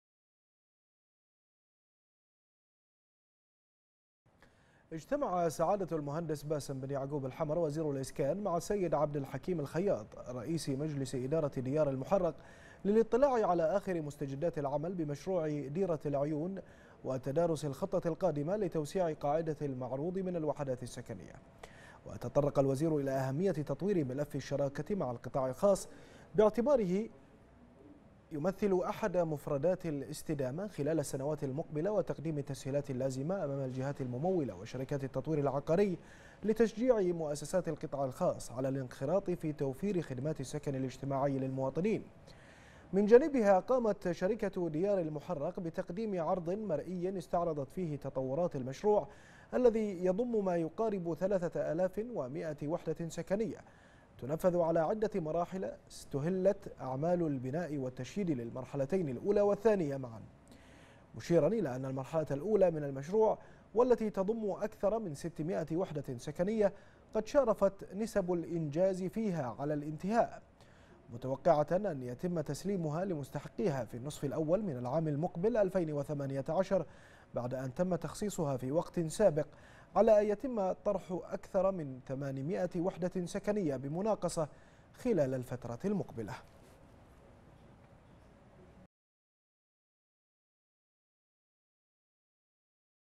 البحرين : وزير الاسكان يطلع على اخر مستجدات مشروع "ديرة العيون"
وزير الاسكان يطلع على اخر مستجدات مشروع "ديرة العيون"